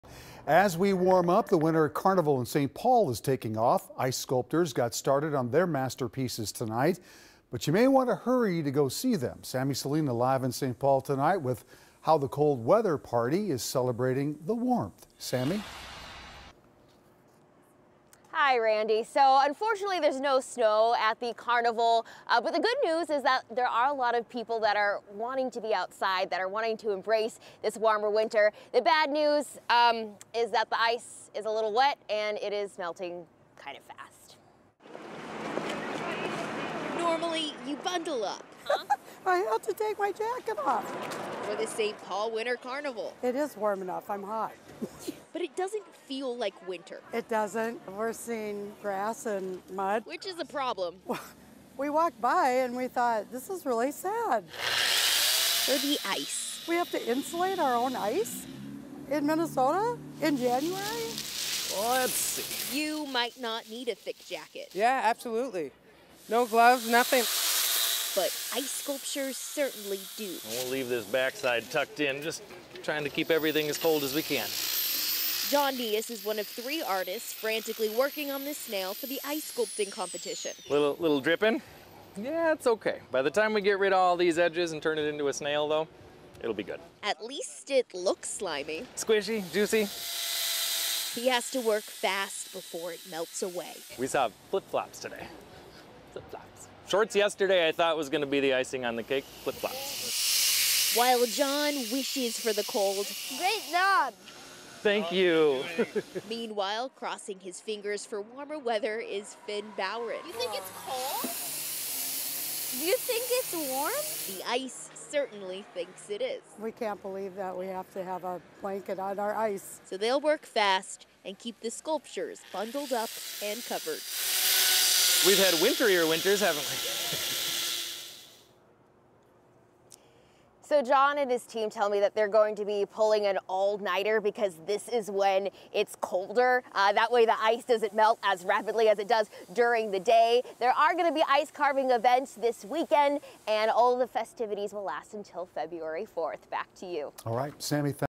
Minnesotans make the best of Saint Paul Winter Carnival despite the lack of winter
You might not need a thick jacket, but the ice sculptures might.